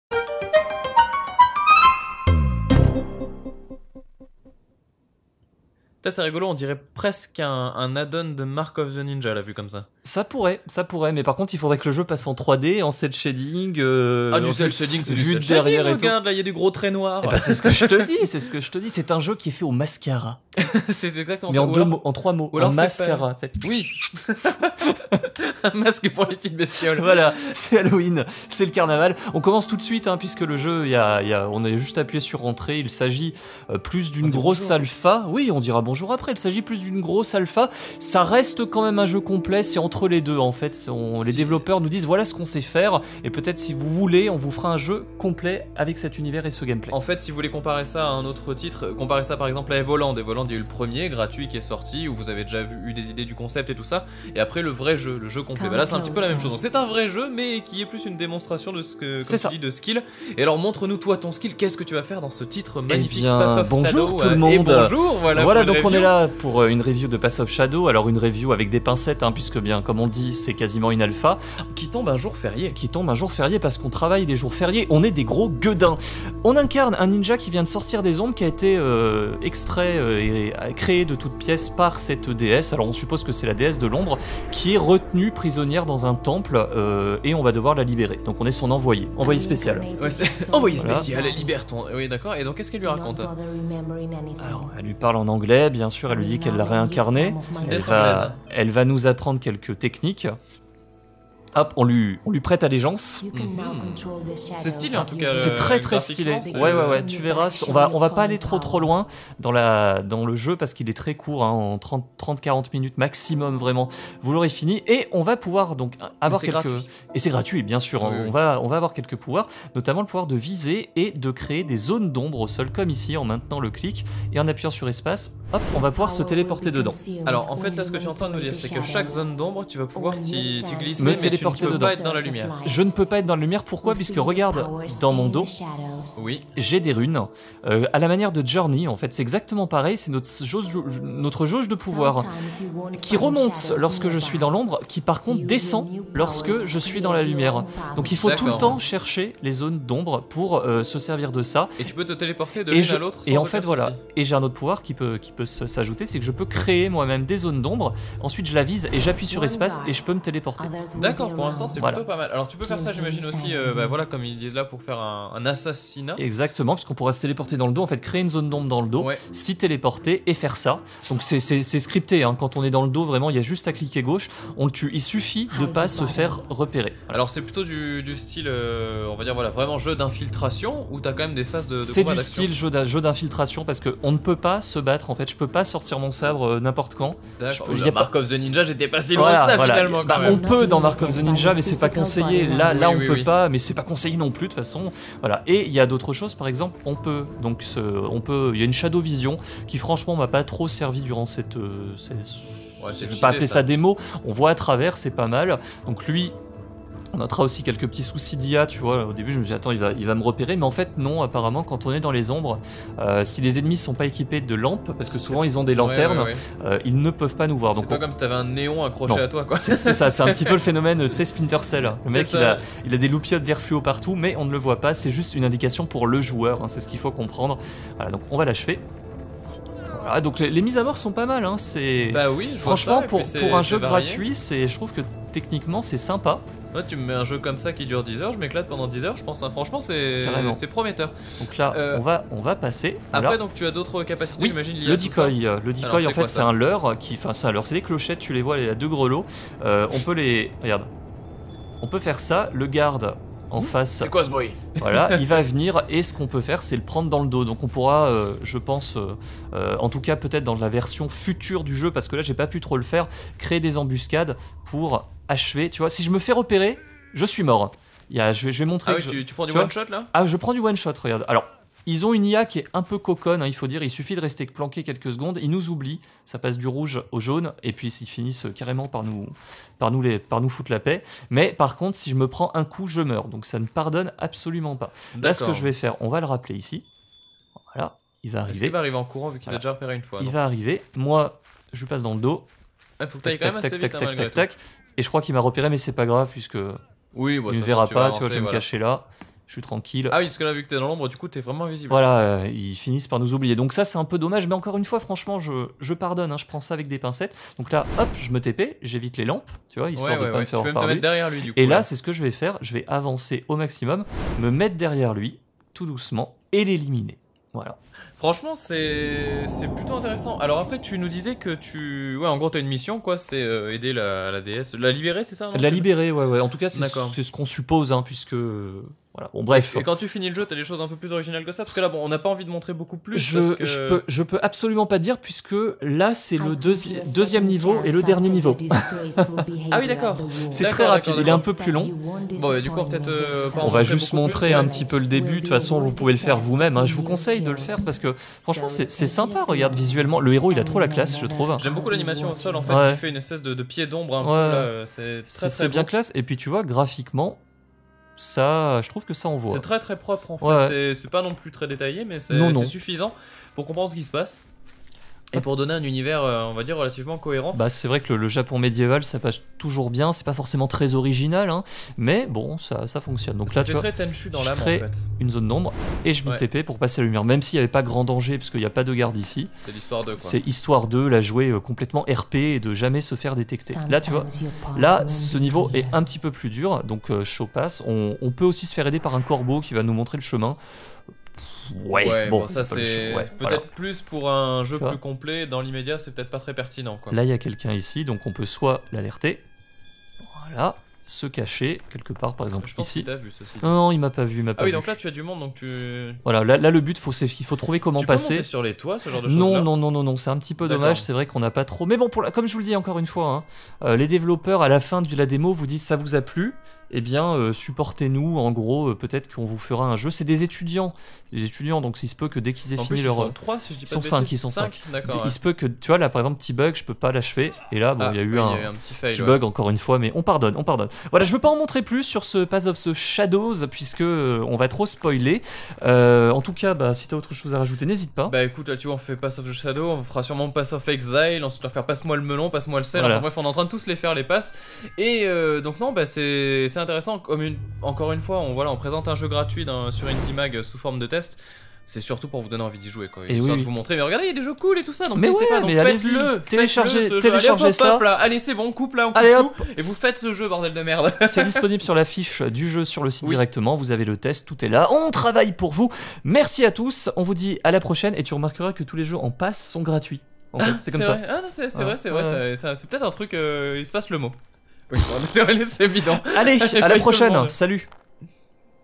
Indie Review - Path of Shadows (PC)
Du gratuit comme ça on en redemande ! Même si la version actuelle de Path of Shadows n'est peut être qu'une présentation d'un futur jeu plus long et plus varié, il saura toutefois satisfaire les amateurs d'infiltration à l'ambiance très sombre. Le personnage principal dégage une classe incroyable et ses capacités ne demandent qu'à s'exprimer plus en profondeur dans une future version du jeu plus longue. Quoi qu'il en soit, il serait bête de passer à côté de ce titre tant son ambiance et ses qualités graphiques le font sortir du lot des productions indépendantes distribuées gratuitement. À vous de jouer, et bon séjour dans les ombres !